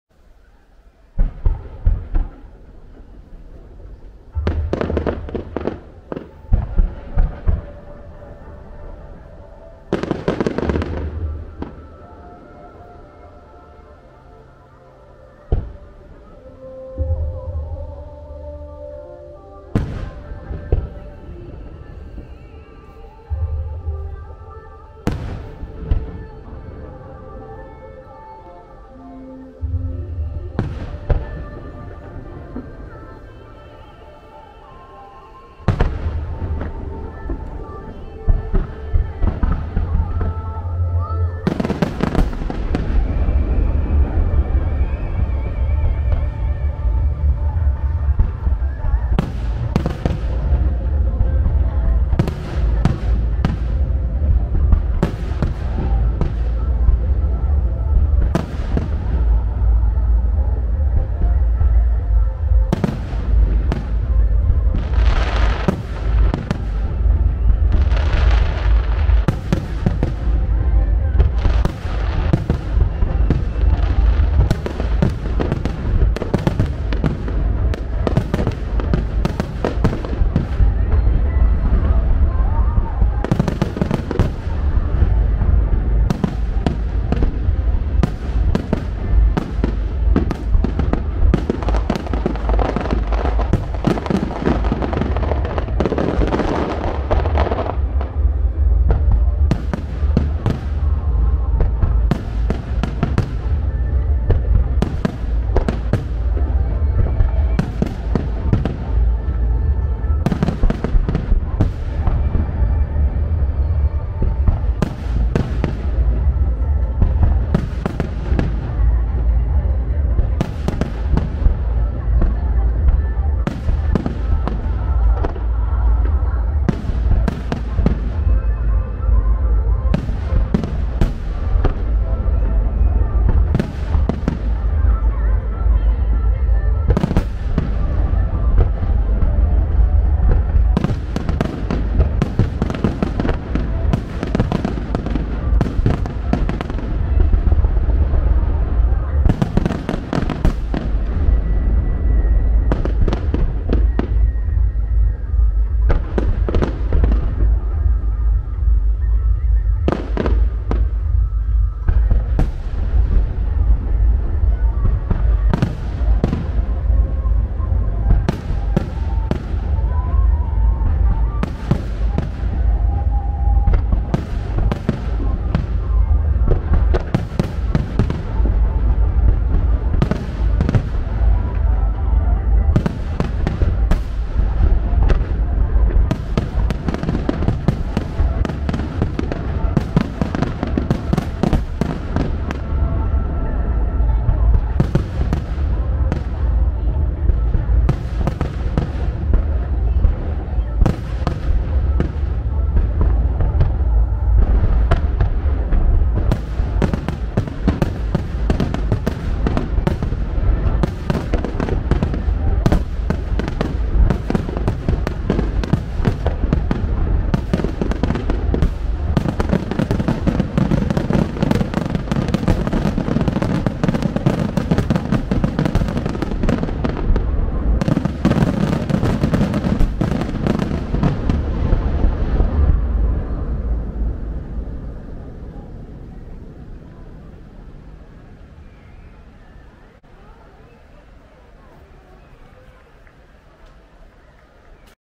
2023.7.29の花火大会フィナーレ
水戸花火大会フィナーレ「千波湖を彩る火の芸術」
超ワイド特大ミュージックスターマインです。